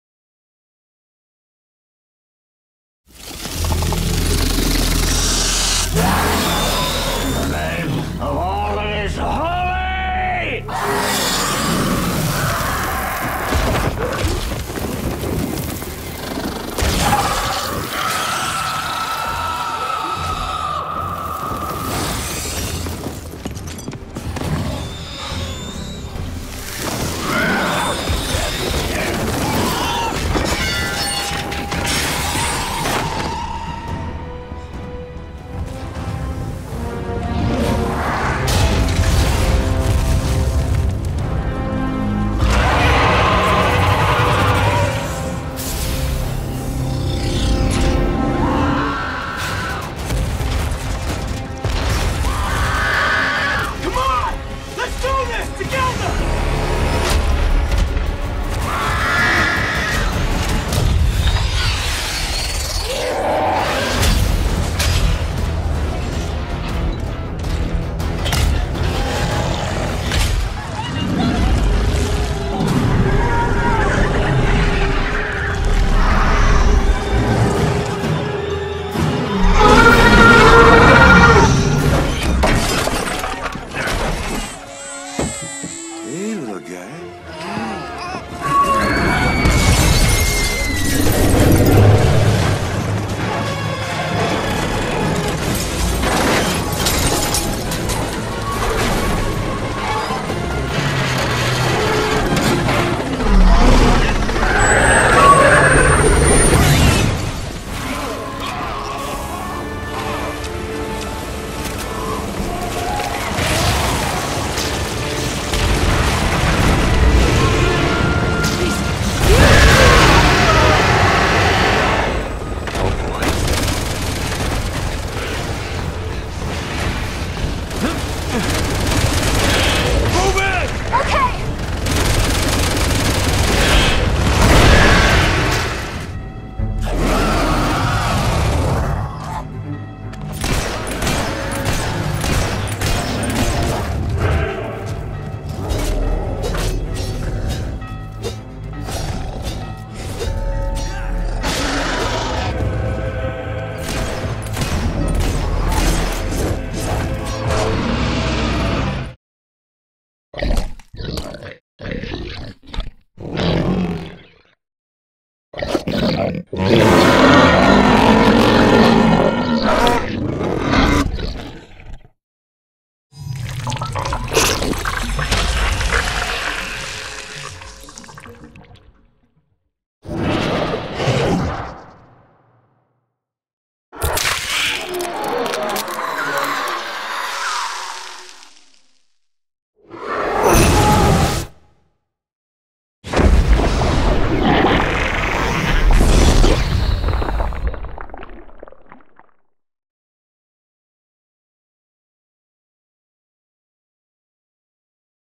Sound Lab - Creature Reel 01
A sample of some of our Creature sound design work in both video-games and film.